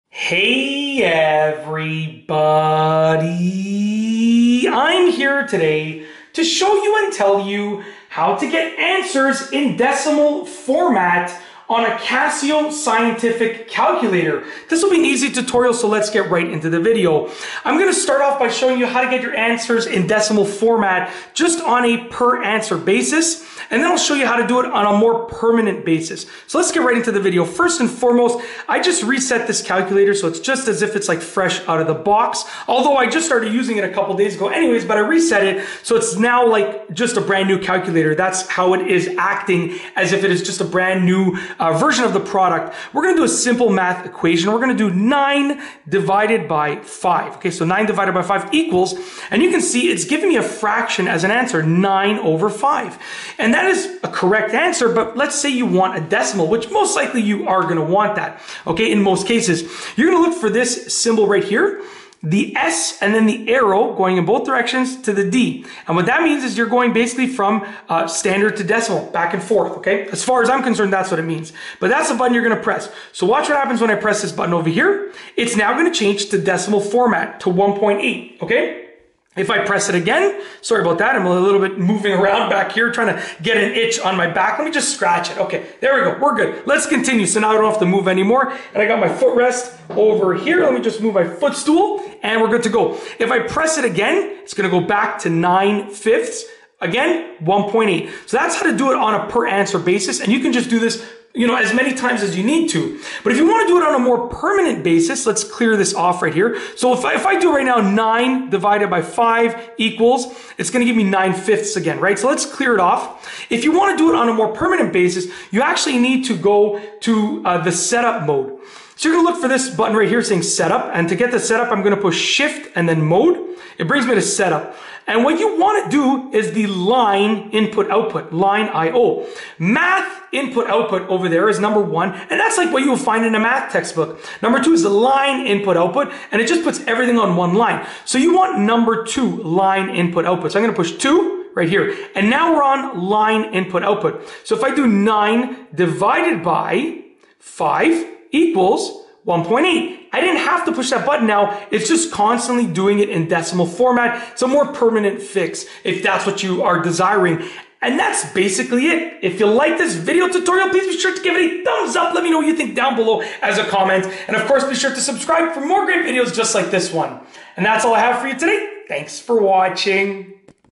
How To Get Answers In Decimal-Casio Scientific Calculator-Tutorial
In this video, I show you and tell you how to get answers in decimal format on the Casio scientific calculator that I have. it is actually a really simple process. I go over the basics of getting decimals in a one-off manner for an individual answer that you want to change from a fraction to a decimal, and then show you a more permanent way so that all of your answers show up in decimal format on the Casio calculator.